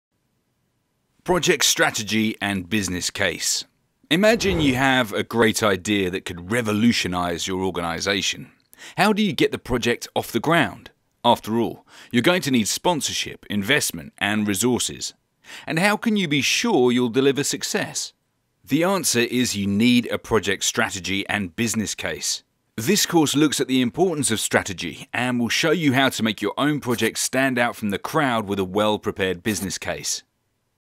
Project Strategy and Business Case Trailer
This course looks at the importance of strategy and will show you how to make your own projects stand out from the crowd with a well prepared business case.

Call Learning Heroes on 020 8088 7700 to gain unlimited access to this course today.